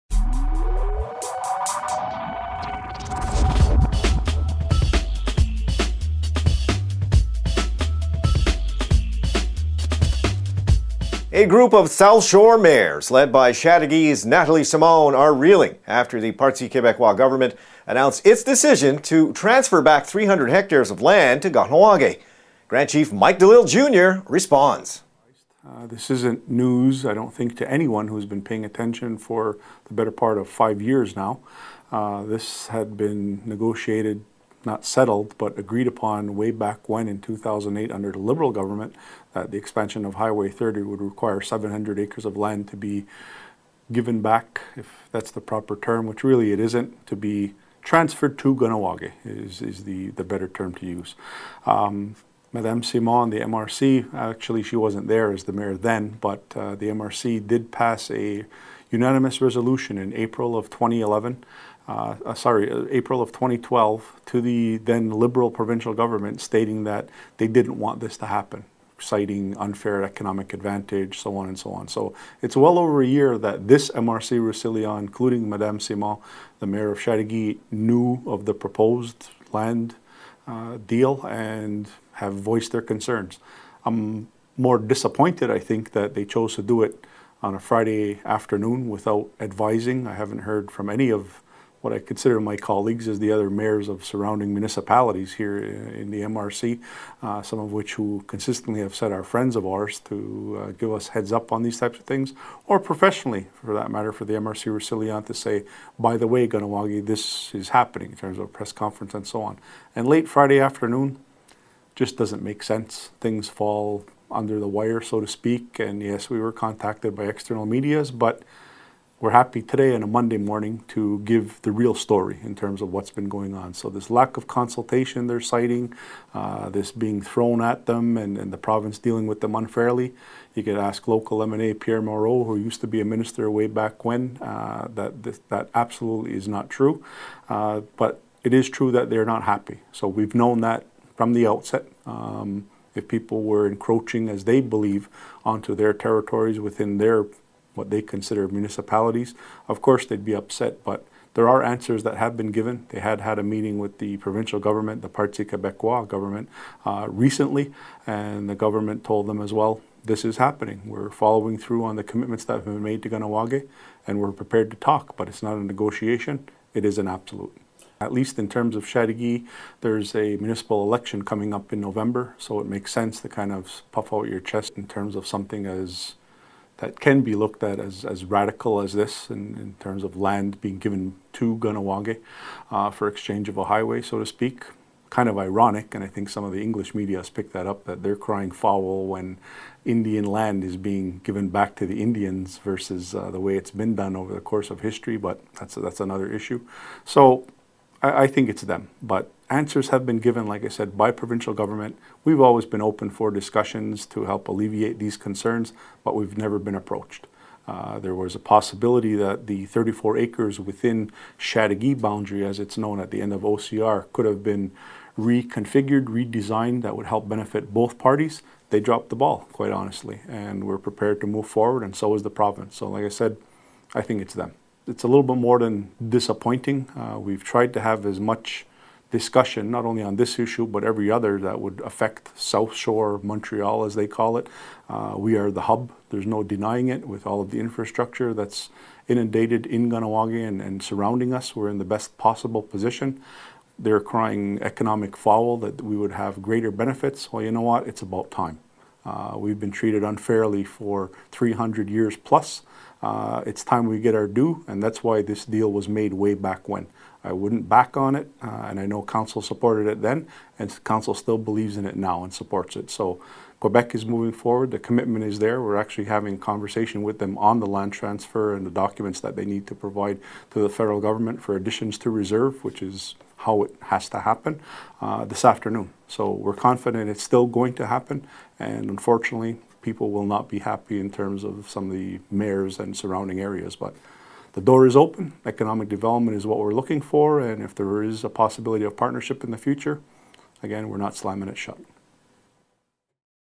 Grand Chief Responds to 'Angry Mayors'
A group of South Shore mayors led by Chateauguay's Nathalie Simon are reeling after the Parti Québécois government announced its decision to transfer 300 hectares of land to Kahnawà:ke. Grand Chief Mike Delisle Jr. responds.